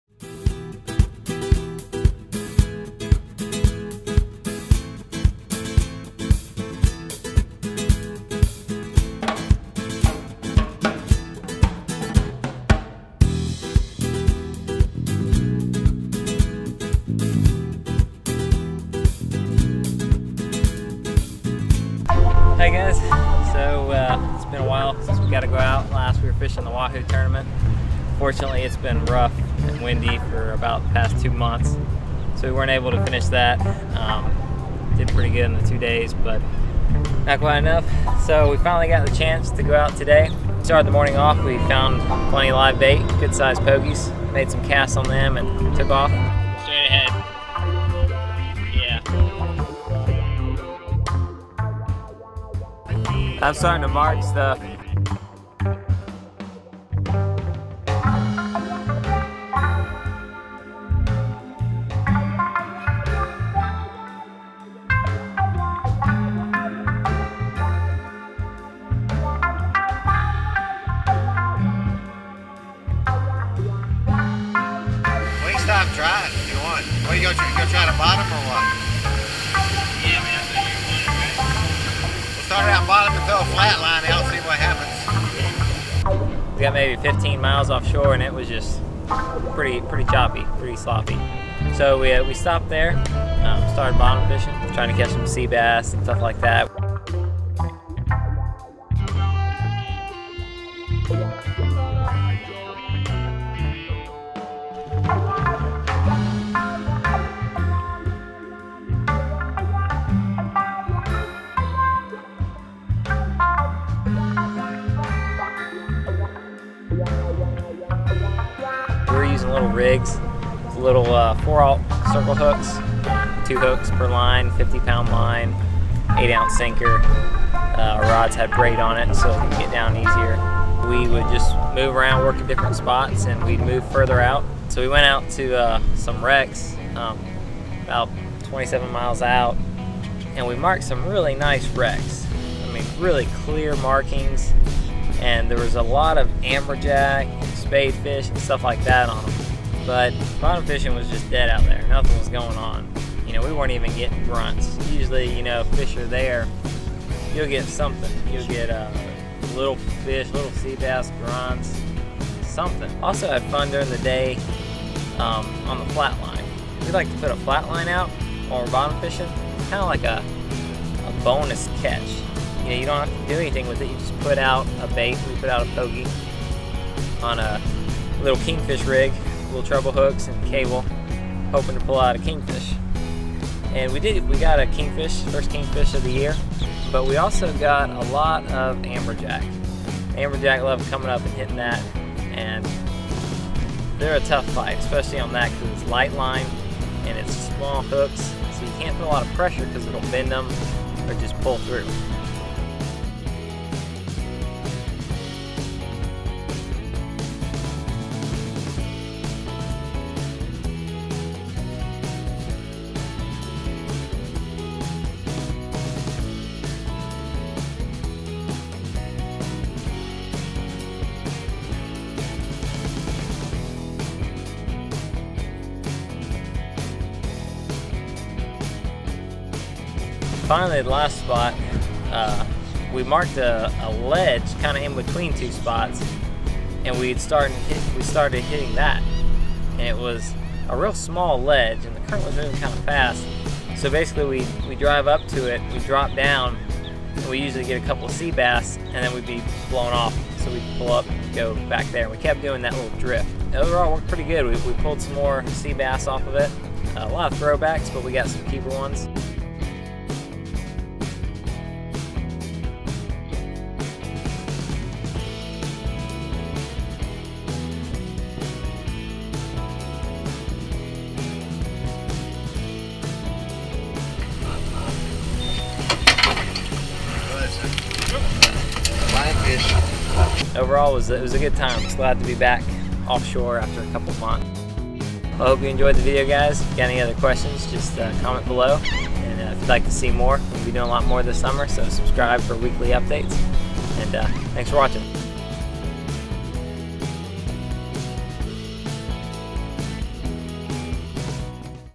Bottom Fishing Trip: Sea Bass, Amberjack, and Snapper
After a couple months of being land locked we finally make it offshore! Strong winds and rough seas kept us from going far out so we stayed about 18 miles offshore and started bottom fishing. We were using cut pogeys to catch Sea Bass, Snapper, Amberjack, and Kingfish. Even though the bite wasn't as hot as we had hoped it was a fun day offshore!